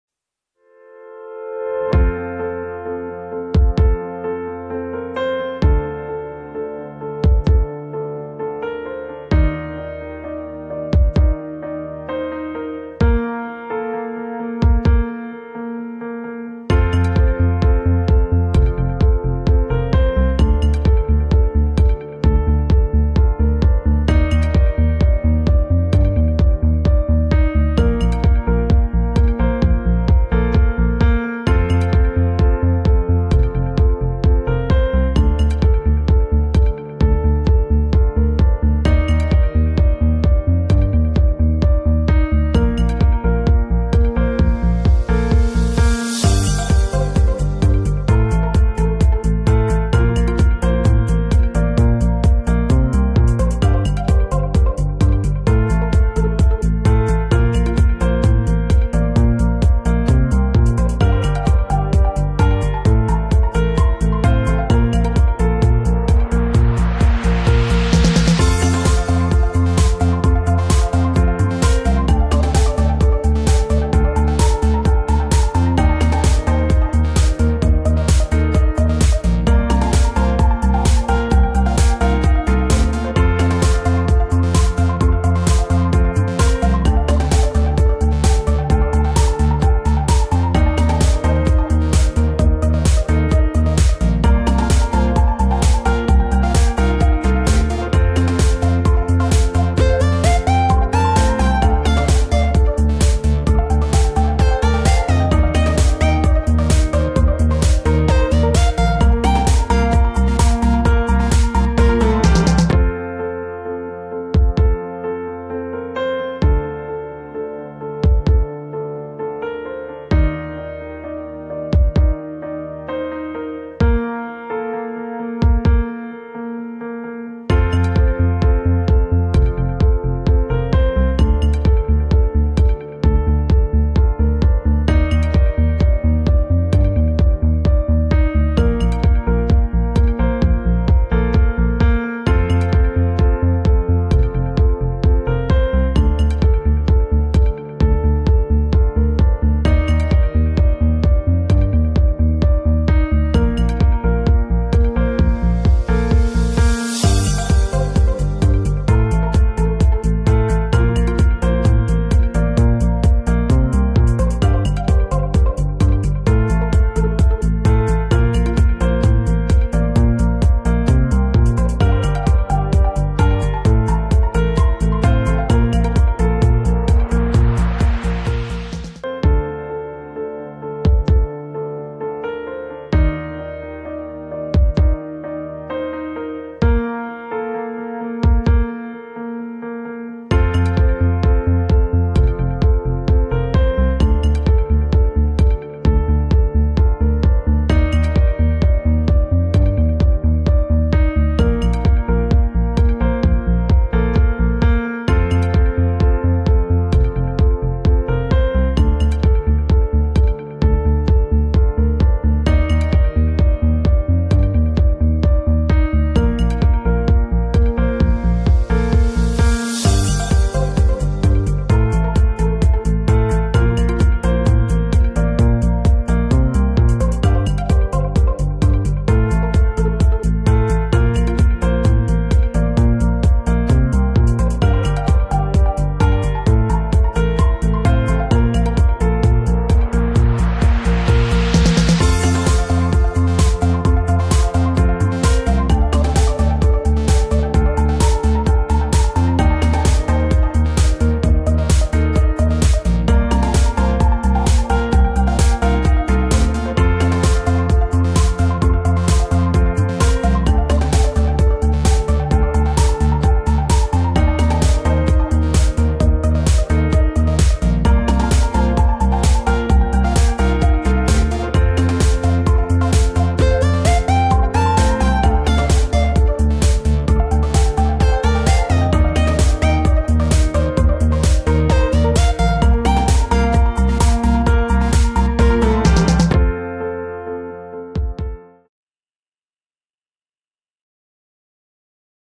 Zambia Geography ... Country In a nutshell
We are here again with another country episode. And today we are from the Friendly nation of Zambia.Country with a lots of cool lake, majestic water falls and mighty Zambezi . Lets know the country a little more...Sorry for the spelling and grammar mistakes in the video.